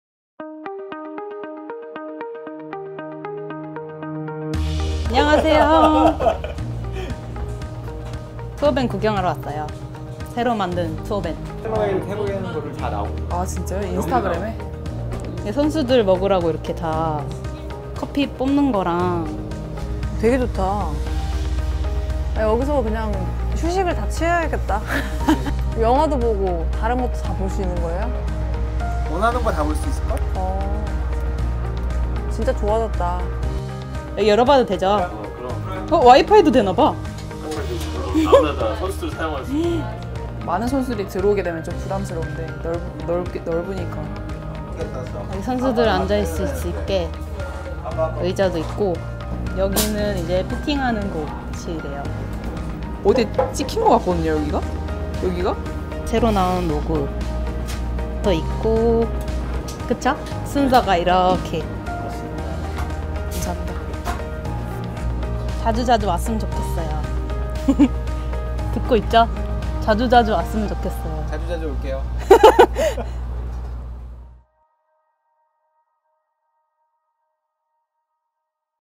[캘러웨이] 2018 신형 투어밴 : KLPGA 투어
▶ 14톤에 이르는 최대의 규모
▶ 최고급 멀티미디어 시스템
▶ 클럽 전문가들의 피팅 서비스를 제공하는 기어 팩토리
▶ 비디오 게임과 휴식을 즐길 수 있는 플레이 테라스
▶ 선수 상담 및 지원 물품을 상비한 오퍼레이션 룸

2018 투어에서 선수들을 완벽하게 서포트할 캘러웨이의 새로운 투어밴을 영상으로 만나보세요.